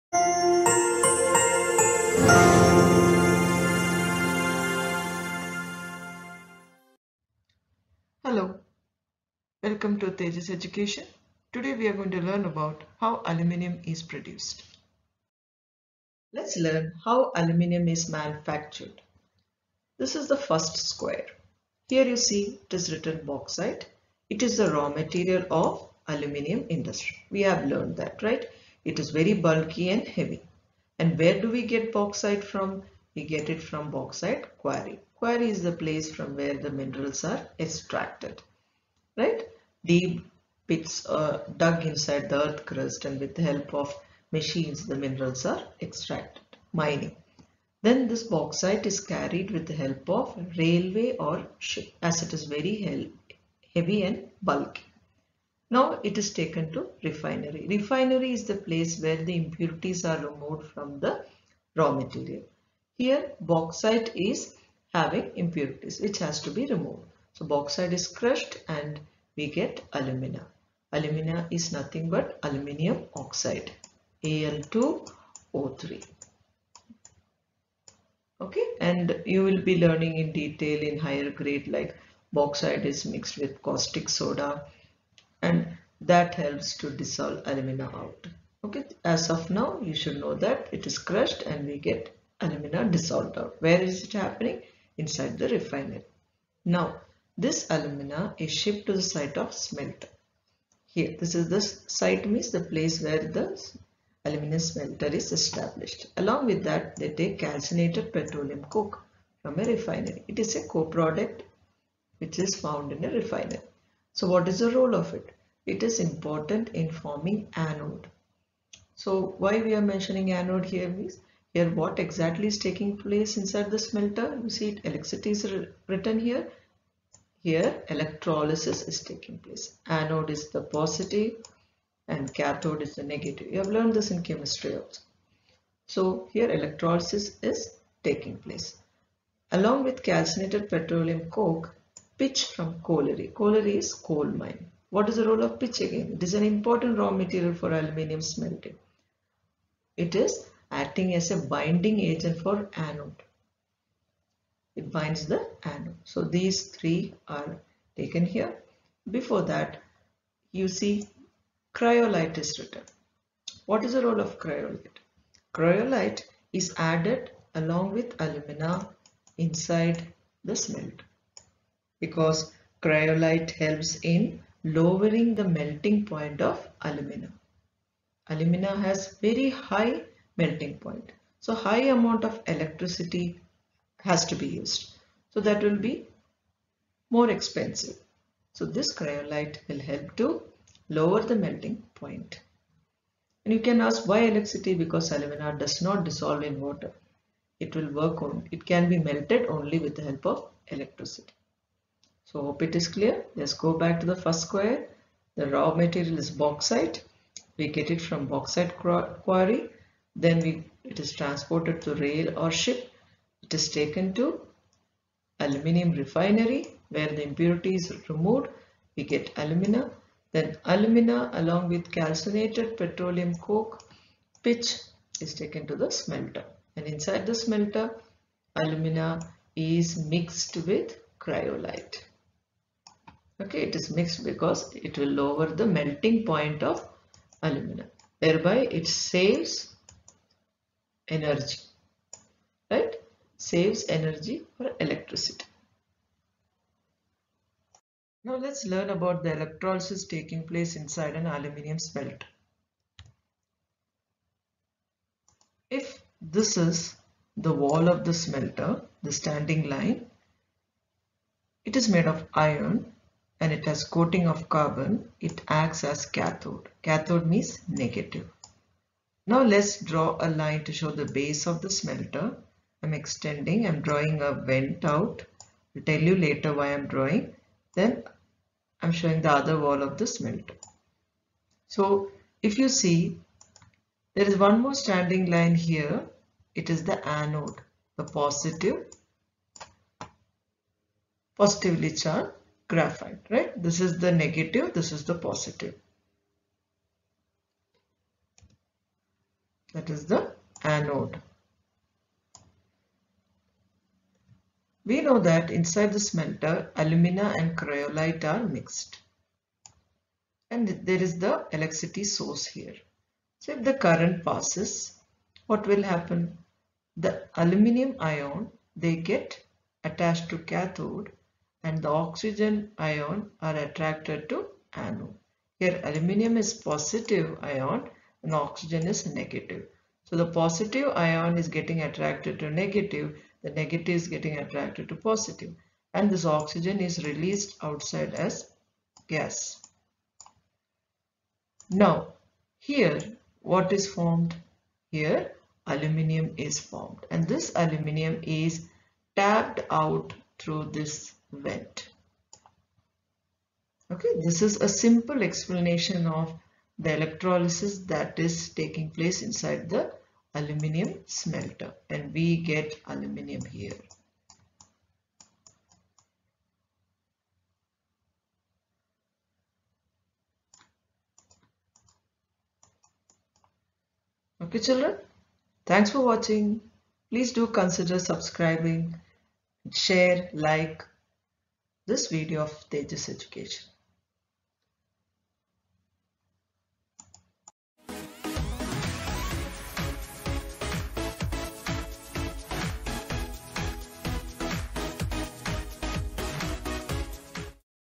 Aluminum | Manufacturing industries| Class 10 Geography CBSE
This video of TejusEducation explains how aluminium is manufactured inside the aluminium smelter.
Bauxite is the raw material for Aluminium Industry.It is very heavy and bulky.It is transported to Aluminium refinery with the help of railway and ship .The bauxite is crushed and alumina is dissolved out in the aluminium refinery.Then this alumina is transferred to aluminium smelter.along with that calcined petroleum coke and pitch from colliery is transferred. Calcined petroleum coke is required to form anode and pitch is an important raw material to bind anode together.
Inside smelter the Alumina and Cryolite is mixed ,Cryolite acts as an electrolyte.It lowers melting point of Alumina and saves energy.
how the electrolysis takes place inside Aluminium Smelter?
The wall of the smelter acts as a cathode as it is made of iron and has a coating of carbon and anode is made of calcined petroleum coke and pitch binds the anode.When current passes through them ,the aluminium ion which are positively charged get attached to cathode and oxygen ion which are negative get attached to anode .The oxygen is released outside.The aluminium is formed at cathode is tapped out at regular interval.
Please do watch the entire video
Please watch Lesson Manufacturing Videos

11) Informational Technology and Electronics Industry
12) Industrial Pollution and Environmental Degradation
13) Control of Environmental Degradation